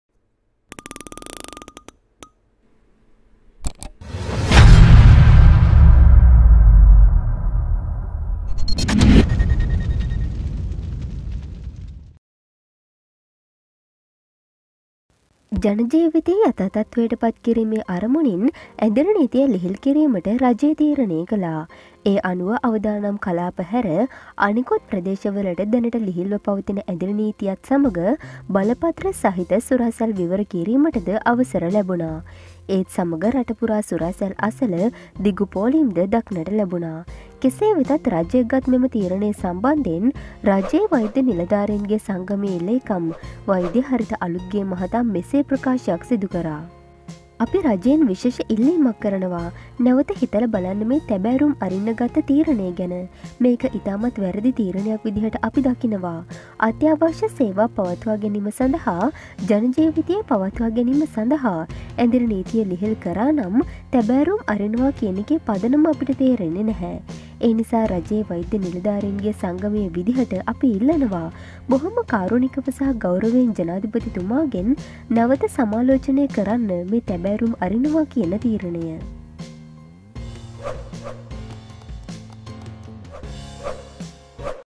එක අතකින් වෛද්‍යසංගමය හරි බං
ජනජීවිතය යථා තත්වයට පත්කිරීමේ අරමුණින් ඇඳිරිනීතිය ලිහිල් කිරීමට රජය තීරණය කළා.

ඒ අනුව අවධානම් කලාප හැර අනෙකුත් ප්‍රදේශවල දැනට ලිහිල්ව පවතින ඇඳිරිනීතියත් සමග බලපත්‍ර සහිත සුරාසැල් විවර කිරීමටද අවසර ලැබුනා.

ඒත් සමග රට පුරා සුරාසැල් අසල දිගු පෝලිම්ද දක්නට ලැබුනා.